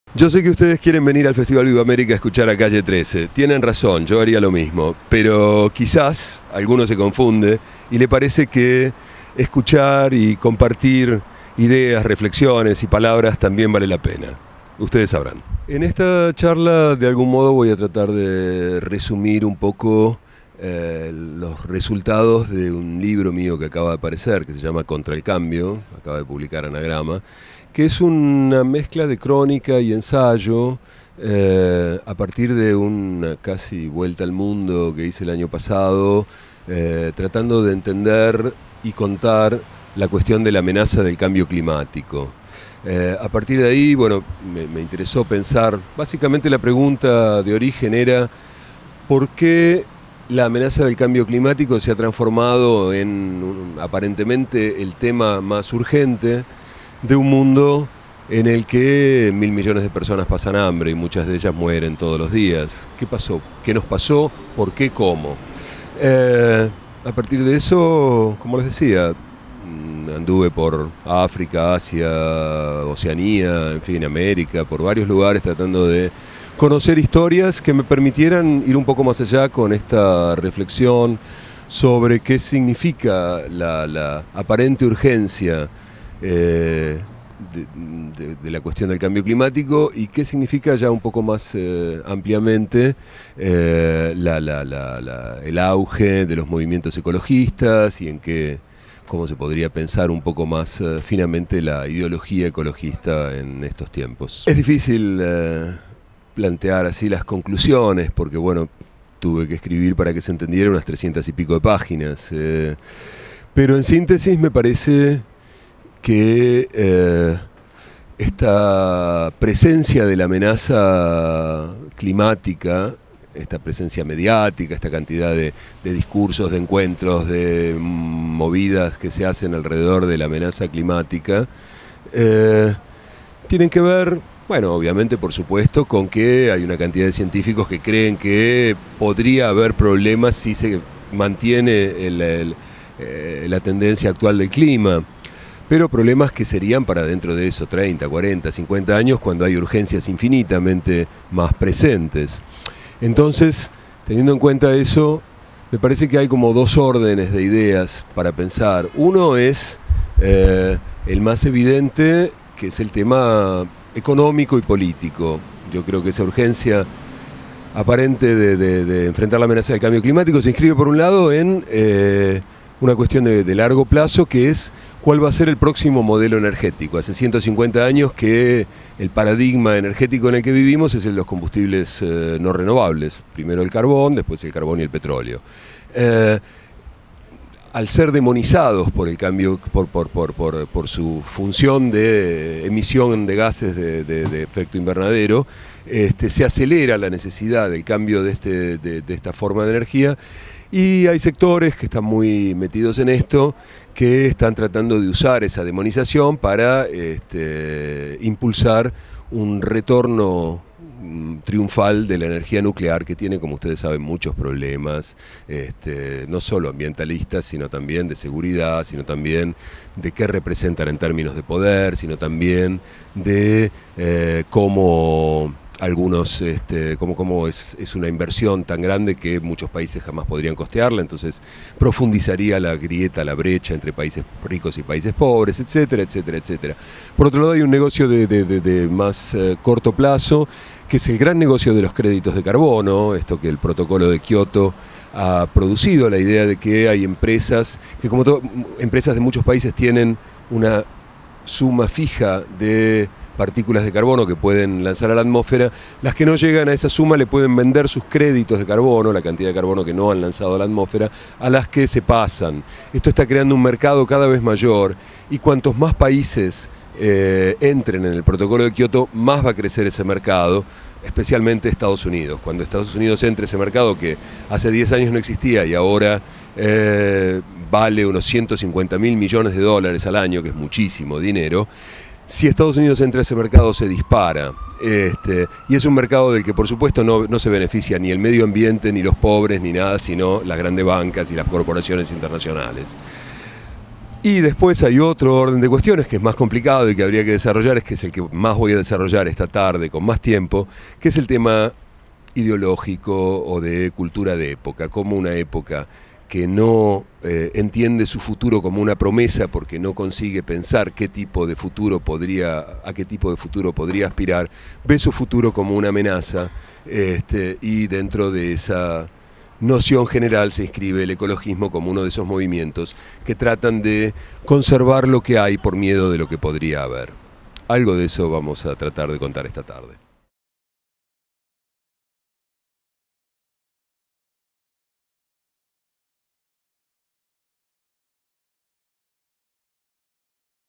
Martín Caparrós: entrevista
El escritor argentino nos habla sobre su conferencia "Qué cambia cuando todo cambia".